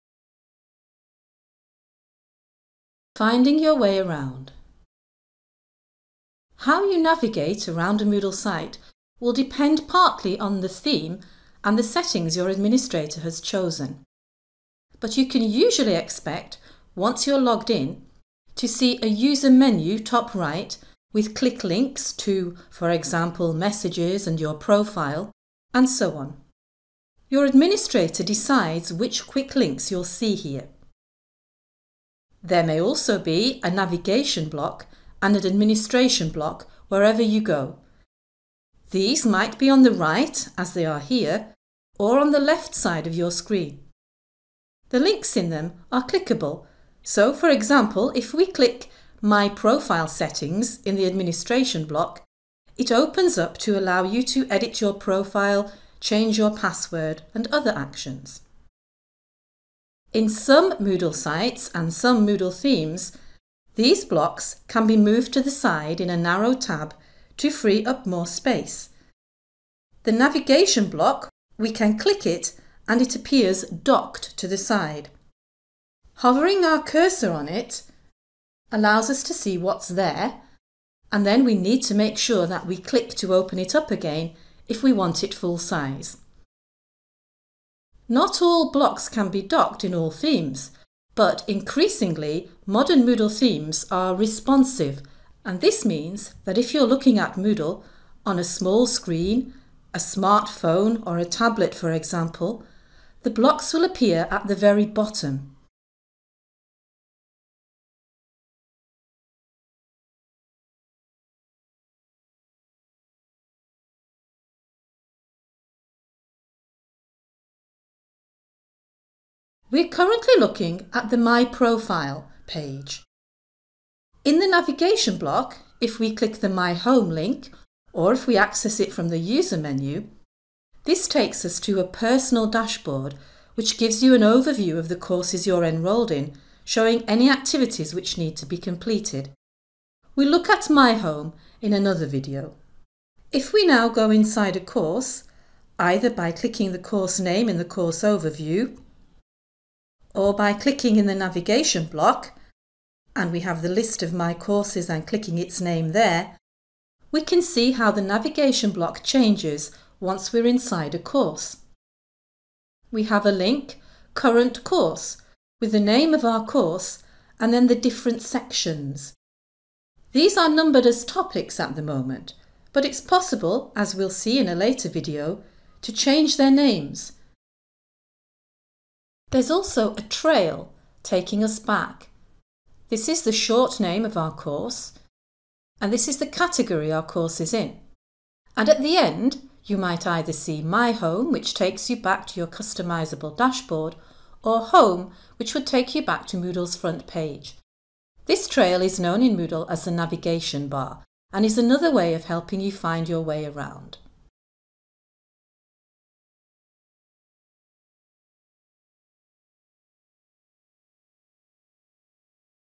Learn Moodle Jan 2015: Finding Your Way Around
How to find your way around the Learn Moodle site - an introduction to the Moodle user interface and navigation.

This tutorial video is part of the Learn Moodle MOOC 2015 series using Moodle 2.8